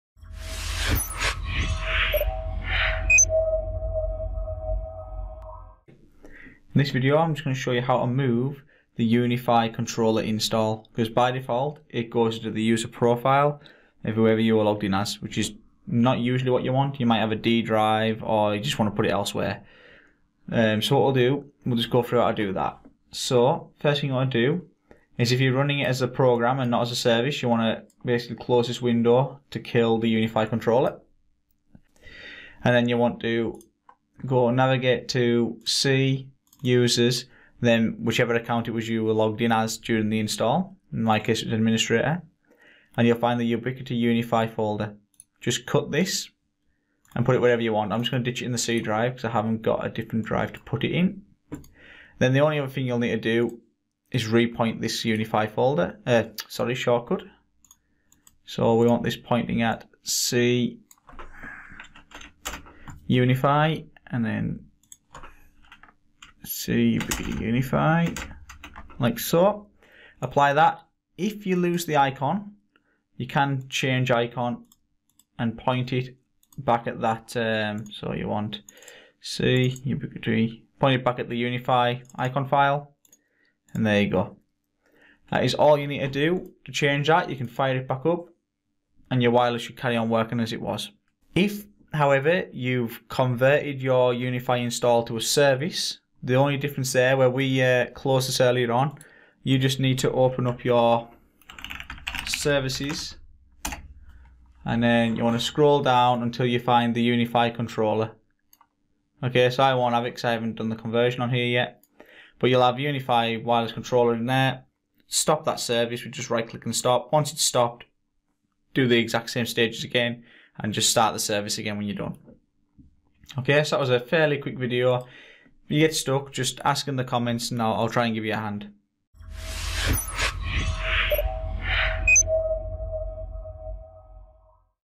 Ubiquiti - Moving the UniFi Controller Install Location!!!
By default the Ubiquiti UniFi controller install directory is the user area of the user logged in during the install. Comeeee on Ubiquiti really... So in this video I show you very quickly how to move that installation after it has been installed!!

It is super easy!